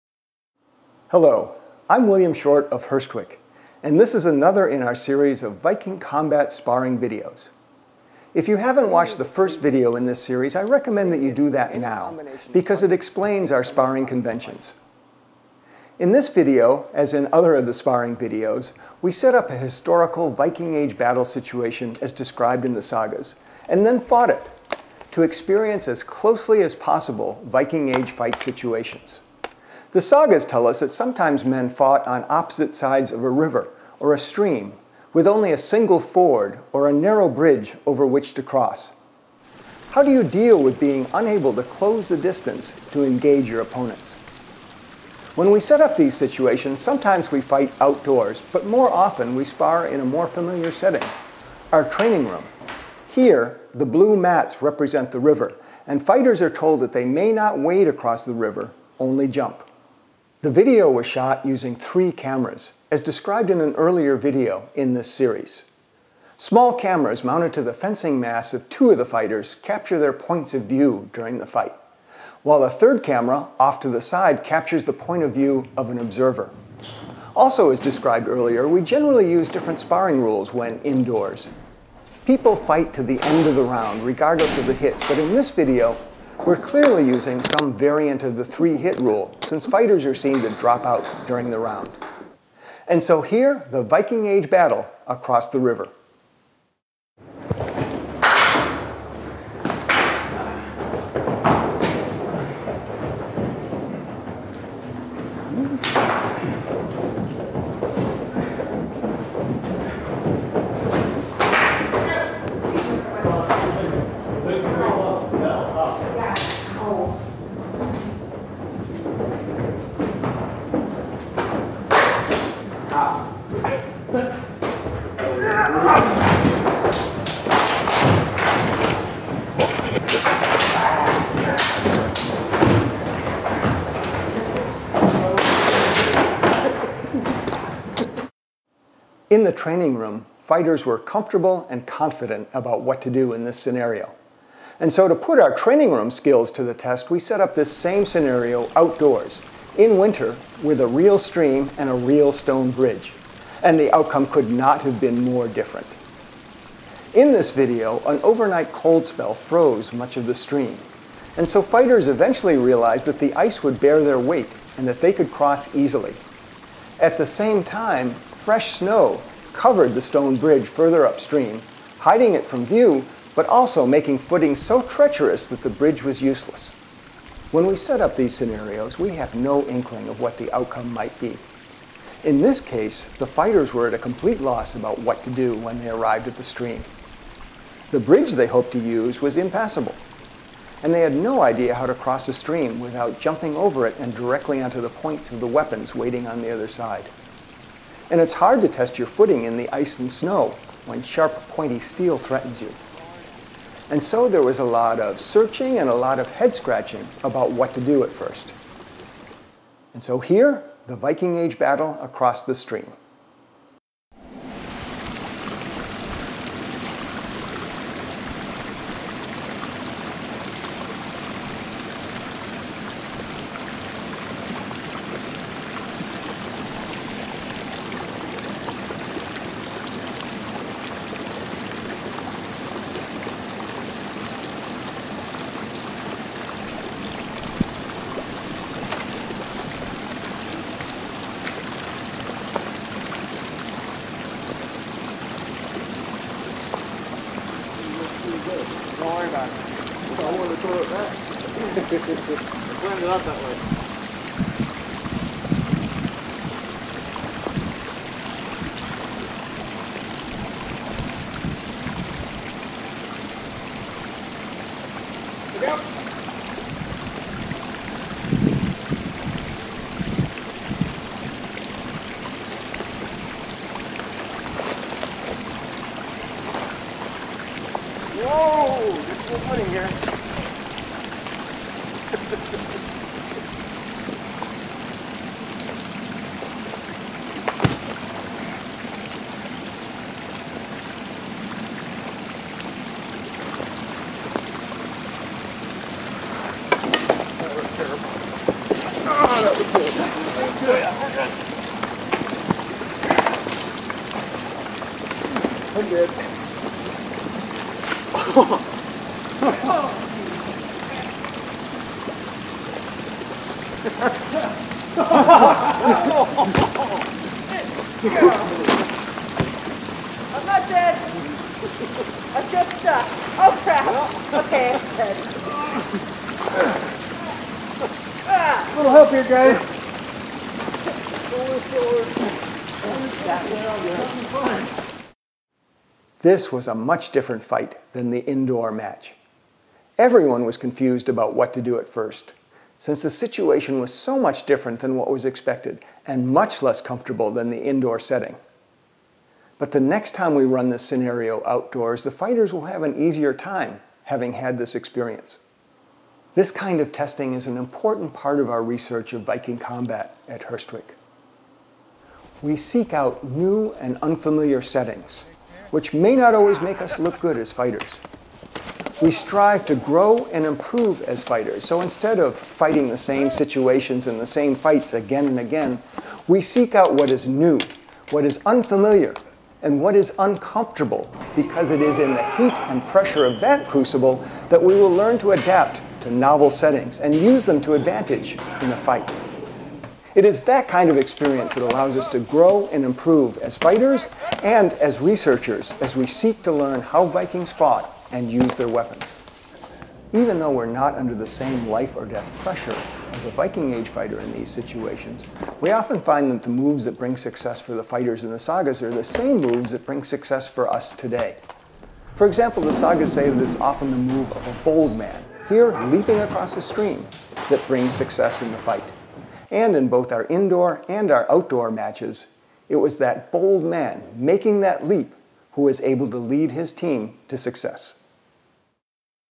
Hurstwic Viking combat sparring exercises 4: river
This is the next in a series of short videos that demonstrate and explain the sparring exercises we use in our Viking combat training at Hurstwic.